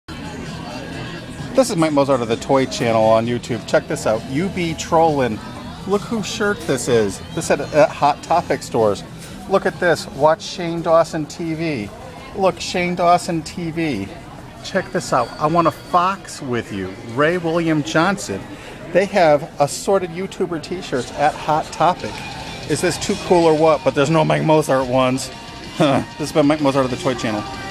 "Shane Dawson" & "Ray William Johnson" T-Shirts at Hot Topic Review by Mike Mozart on TheToyChannel
Mike Mozart IPhone Spy-Cam at Hot Topic where they are selling "Shane Dawson" and "Ray William Johnson" T-Shirts! A Quick Mike Mozart of TheToyChannel on YouTube In Store iphone Spy Cam Video.